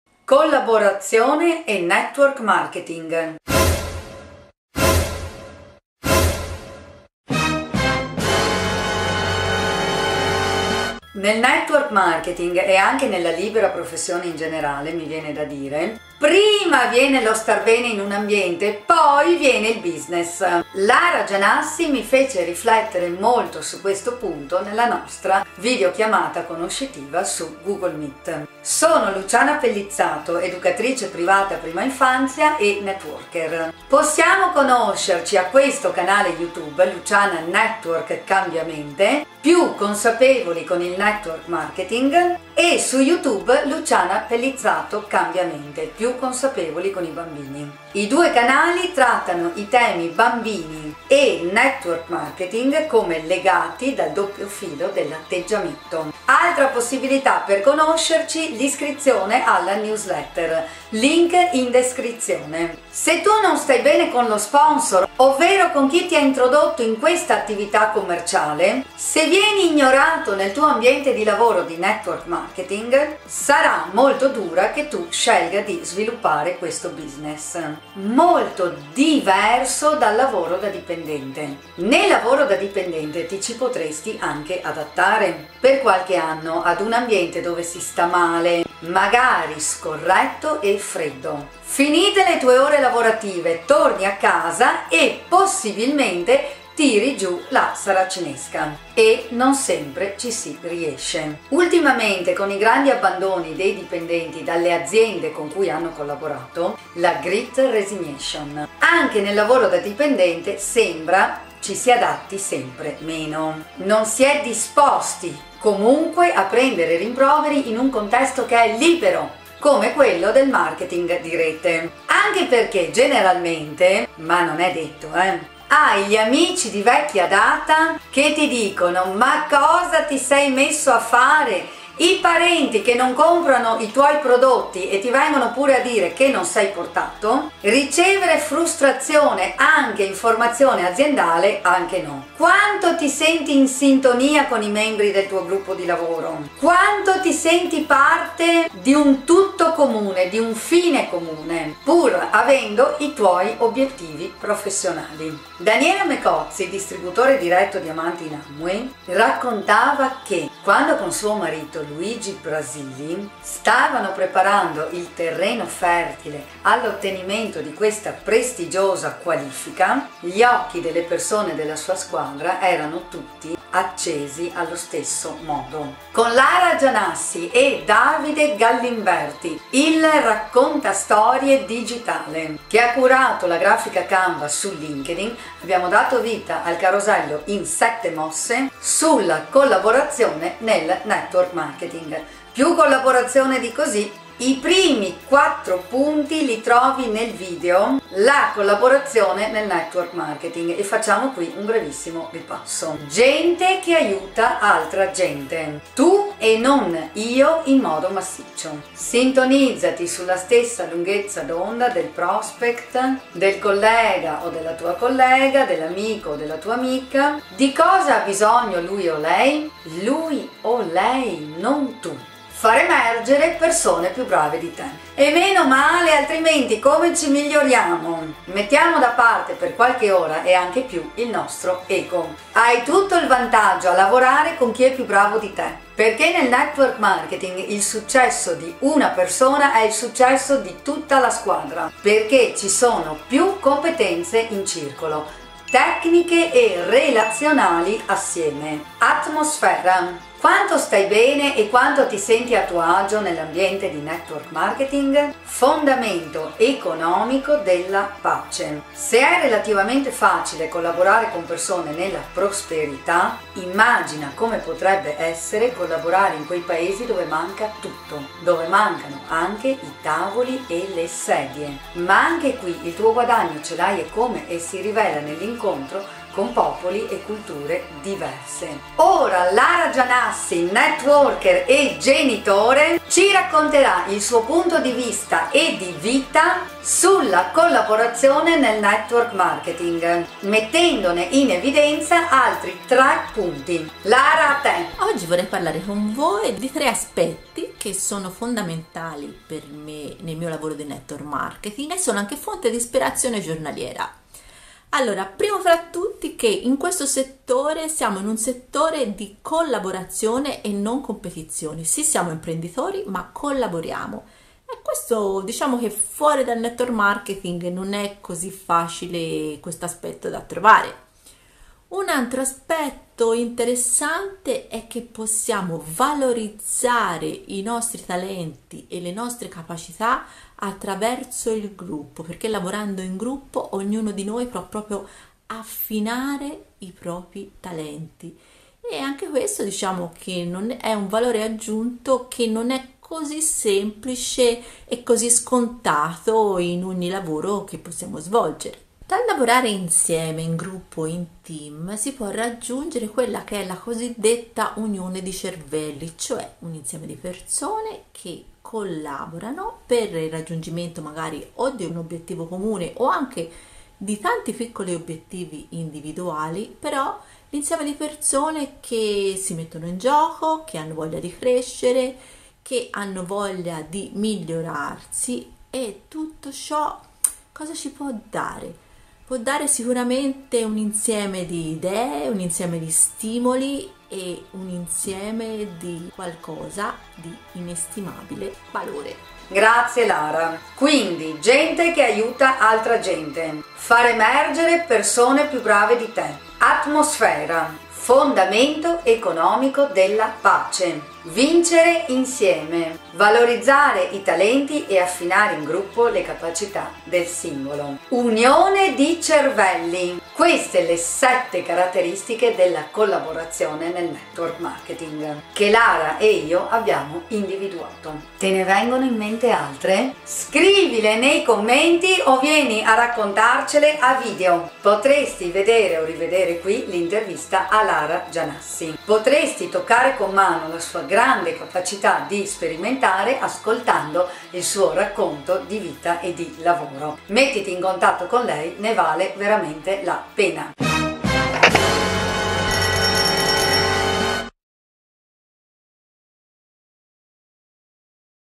COLLABORAZIONE E NETWORK MARKETING CON LARA GIANASSI
📈 I 7 punti della collaborazione nel network marketing. Con Lara Gianassi.
💭 Te ne vengono in mente altri? Potresti scriverceli qui o raccontarceli a video.

Un grazie enorme a Lara Gianassi per la collaborazione 😉 .

Selezione capitoli:
0:00 Collaborazione e network marketing
0:28 Chi sono: colleghiamoci
1:03 Ambiente lavorativo e network marketing
2:56 Collaborazione e network marketing: 7 punti
3:23  1. gente che aiuta altra gente ( Luigi Baccaro )
3:42  2. far emergere persone più brave di te
4:09  3. atmosfera
4:15  4. fondamento economico della pace
4:39  Intervento di Lara Gianassi
5:06  5. No competizione, ma vincere insieme
5:22  6. Valorizzare i talenti e affinare in gruppo le capacità del singolo
5:49  7. Unione di cervelli ( Napoleon Hill )
6:29 Recap e CTA

▶️ Se il video ti è piaciuto potresti mettere 1 like

✅ Network Marketing - Multi Level Marketing - Marketing di Rete sono sinonimi.

Riferimenti bibliografici:
📖 "Network Marketing. Il business vincente nell'era digitale", di Luigi Baccaro, Mondadori Electa S.p.A., Milano 2019
📖 "Il business del 21° secolo" di Robert T. Kiyosaki, 2012 Piero Gribaudi Editore srl
📖 "“Great resignation”: perché è un fenomeno in crescita e come rallentarla", Il Sole 24 Ore, di Gianni Rusconi

🎥 Riprese e montaggio:
     Davide Gallimberti

In questo video è stato usato: trucco e skincare Artistry, cosmesi di prestigio.